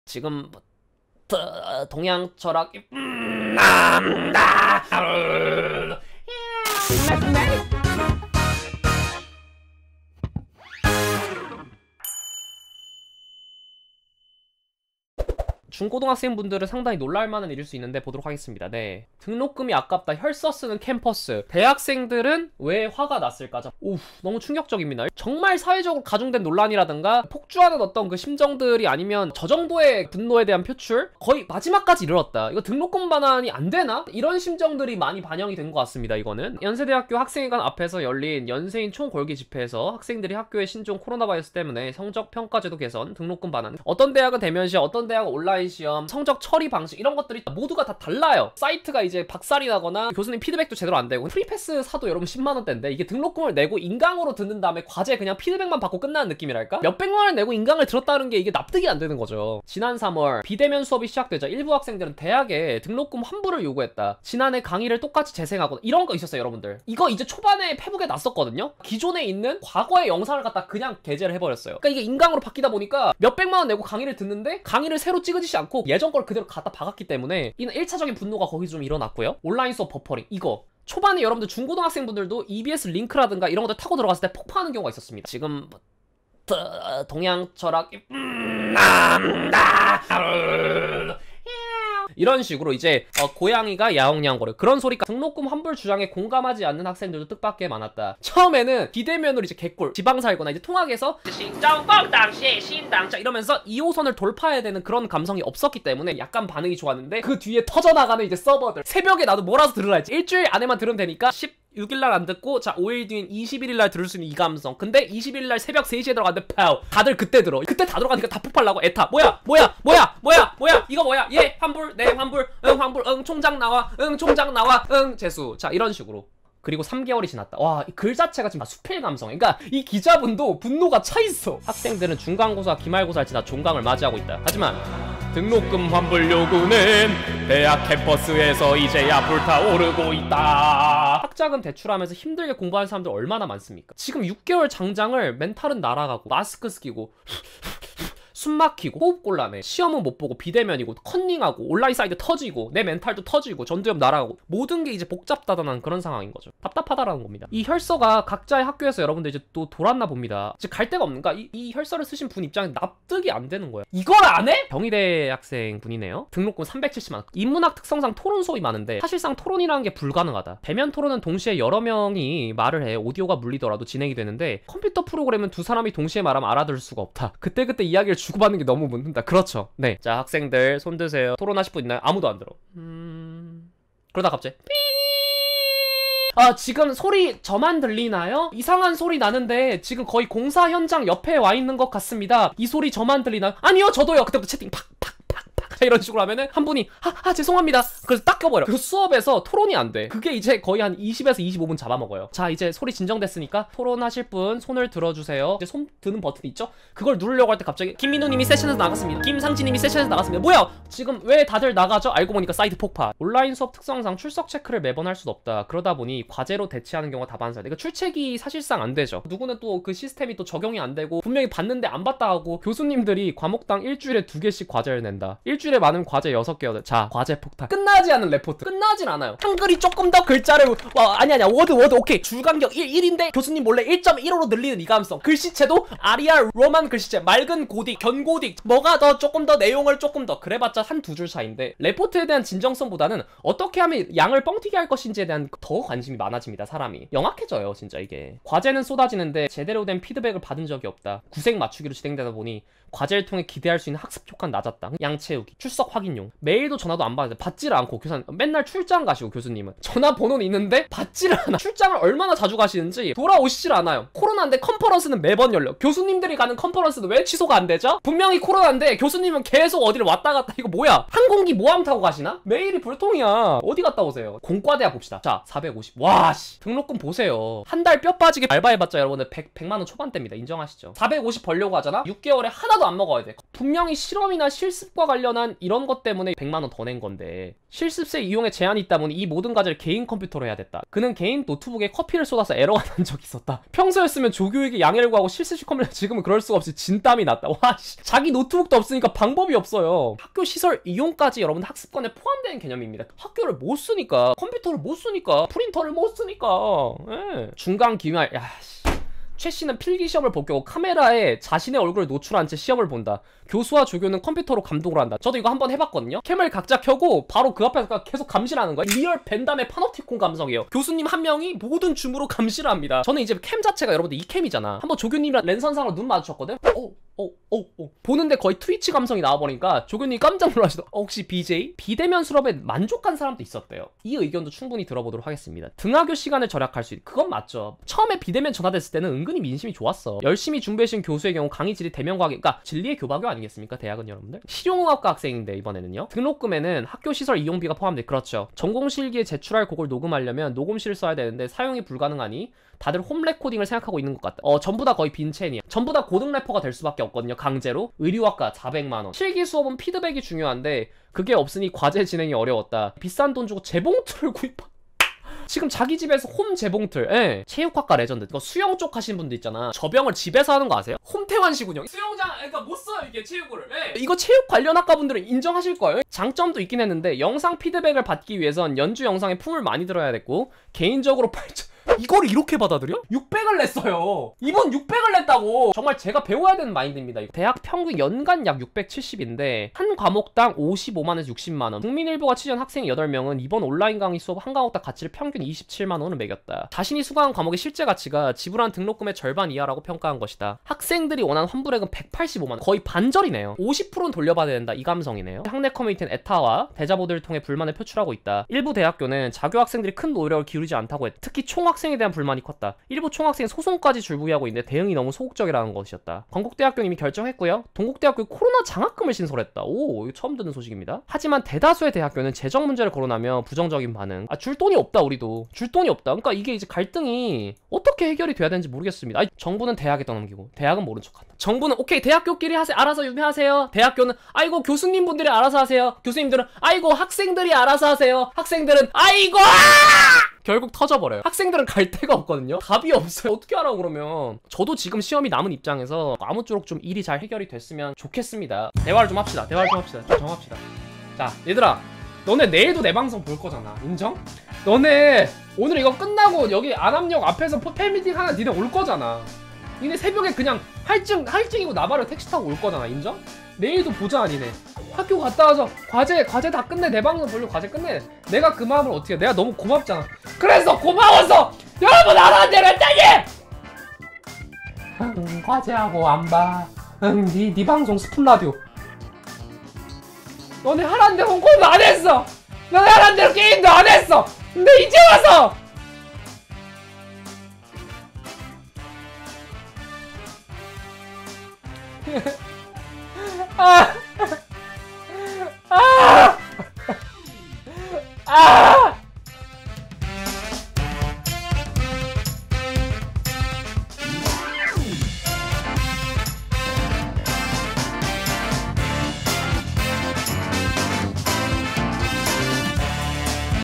거세지는 대학가 갈등..등록금 환불 과연 어떻게 될까요
채널 문의 ▼
미미미누 인스타그램 ▼